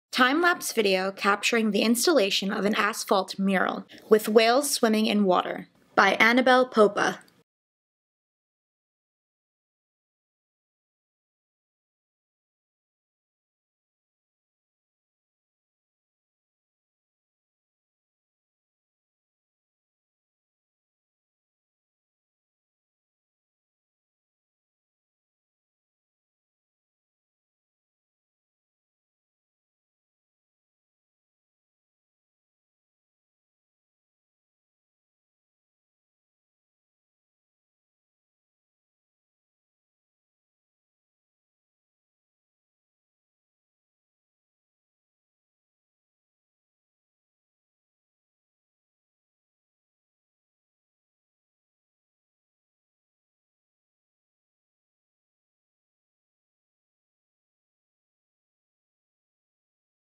DOT Art Asphalt Mural by Annabelle Popa
Timelapse video showing the painting of a new DOT Art asphalt mural in the pedestrianized space across from PS 676 at Columbia Street and Dwight Street in Brooklyn in August 2019.

Learn more about Asphalt Art Activations in NYC: nyc.gov/dotart